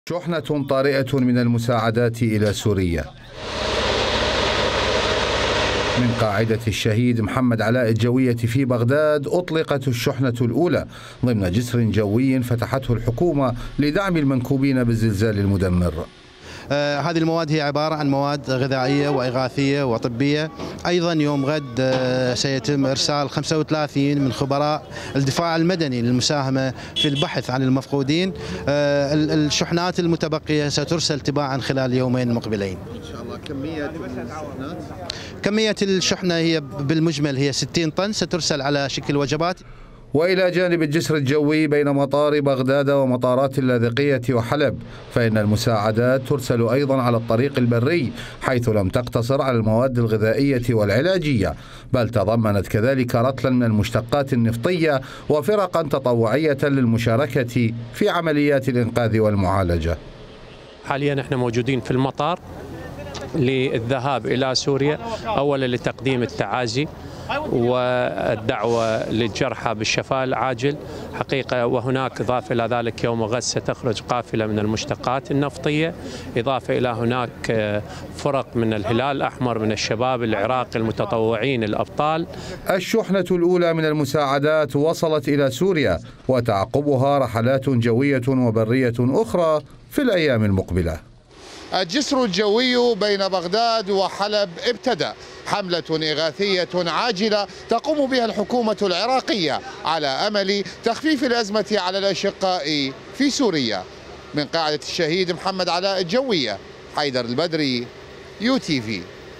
العراق يفتح جسرا جويا مع سوريا لإغاثة المنكوبين بالزلزال.. ومساعدات أخرى تسلك الطريق البري
التردد :-
11354v HD
10727h SD